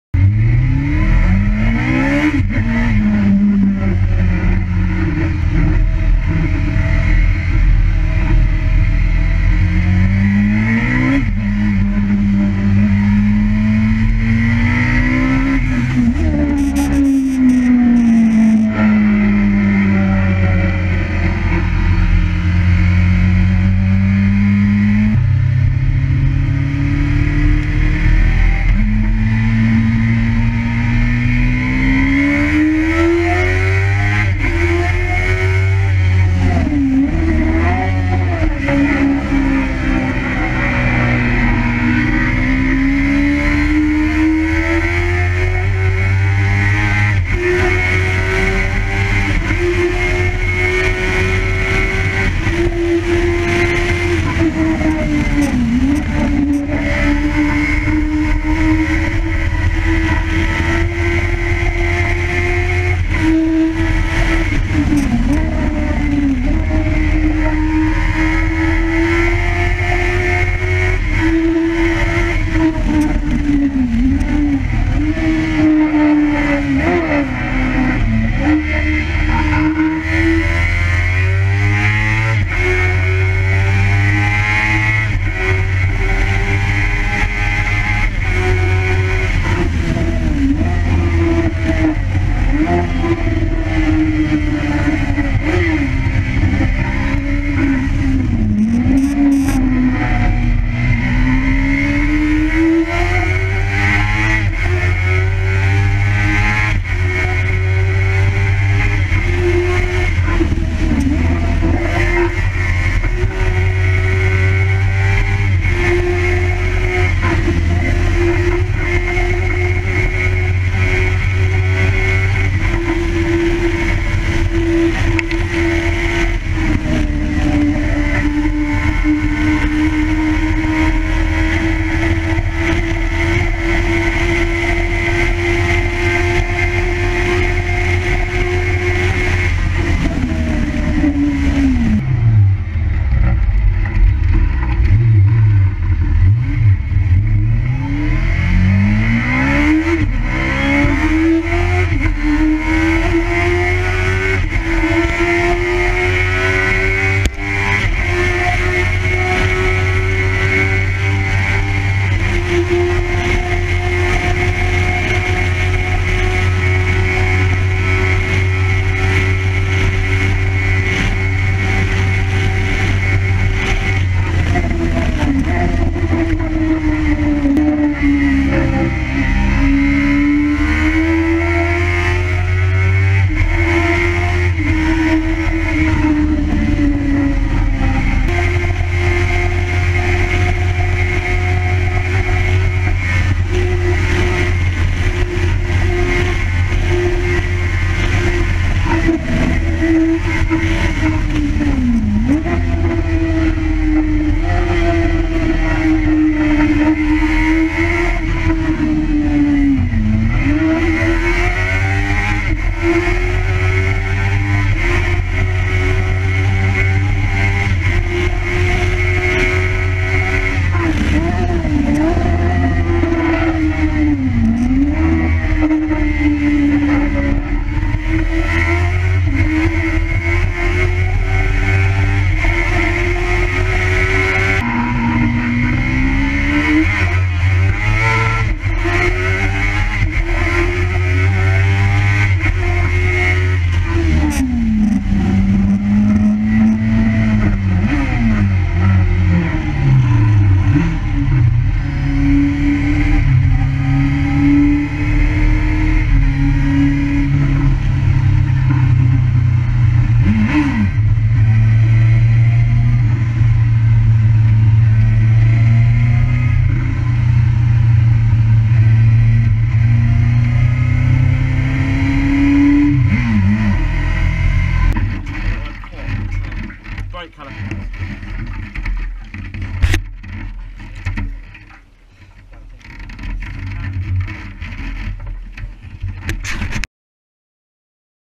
ZX6R top speed run, Brake caliper comes off
Forgot to tighten my calipers, luckily only a broken radiator mesh.